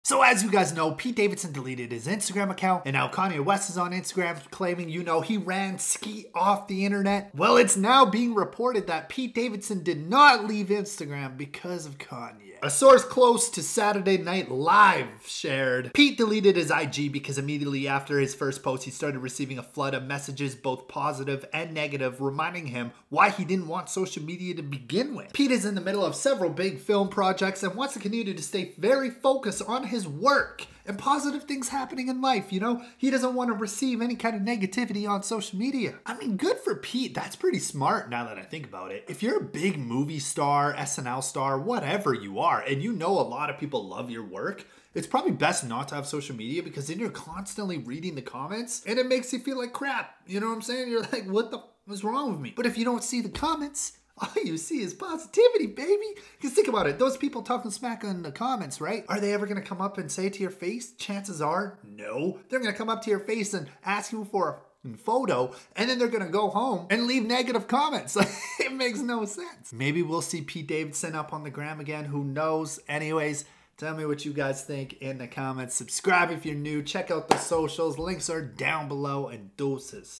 Pete Davidson Reveals Why He DELETED Instagram!!!
sources close to Pete Davidson reveal why he allegedly deleted his Instagram account.. claims it was NOT because of Kanye West.. The SNL star is dating Kim Kardashian and just wants to focus on his movie projects etc